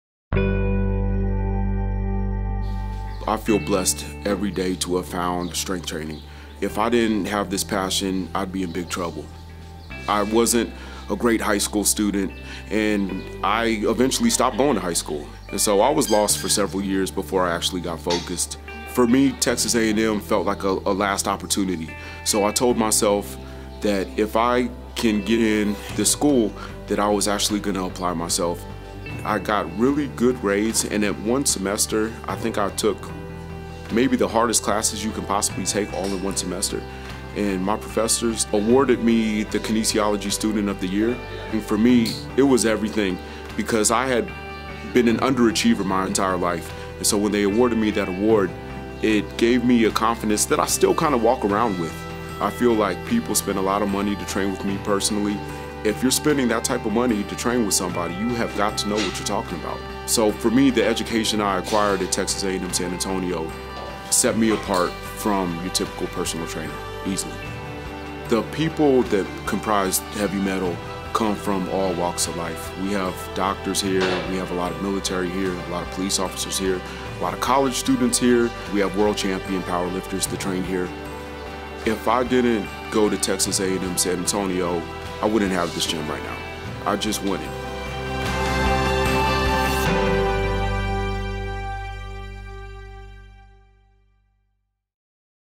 Heavy Metal Fitness | Tim Ingram on his Kinesiology Degree
Tim Ingram of Heavy Metal Fitness shares his inspiring story about getting focused and earning his kinesiology degree from Texas A&M University-San Antonio.

For more, visit: HeavyMetalFit.com
Follow us @HeavyMetalFitness